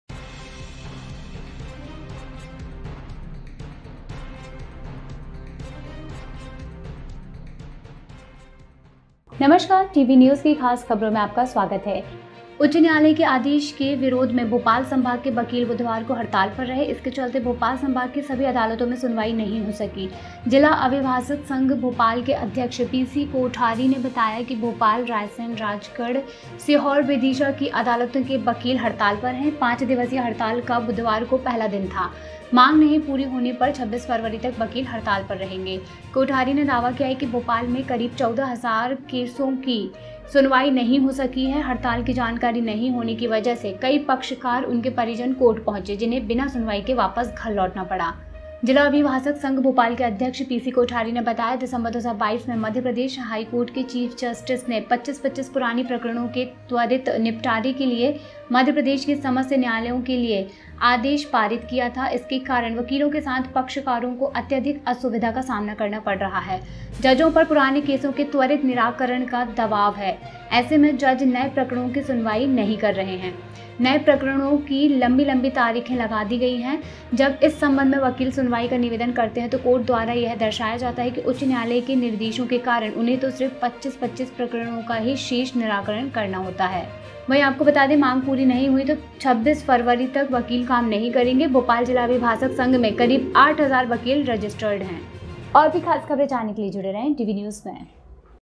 Bhopal: Protest of High Court order | हाई कोर्ट के आदेश का किया विरोध, जानिए यहां | MP
उच्च न्यायालय के आदेश के विरोध में भोपाल संभाग के वकील बुधवार को हड़ताल पर रहे। इसके चलते भोपाल संभाग की सभी अदालतों में सुनवाई नहीं हो सकी। जिला अभिभाषक संघ भोपाल के अध्यक्ष पीसी कोठारी ने बताया कि भोपाल, रायसेन, राजगढ़, सीहोर, विदिशा की अदालतों के वकील हड़ताल पर हैं। पांच दिवसीय हड़ताल का बुधवार को पहला दिन था।

The lawyers of Bhopal division were on strike on Wednesday in protest against the order of the High Court. Due to this, hearing could not be held in all the courts of Bhopal division. PC Kothari, President of District Advocates Association, Bhopal said that the lawyers of the courts of Bhopal, Raisen, Rajgarh, Sehore, Vidisha are on strike. Wednesday was the first day of the five-day strike.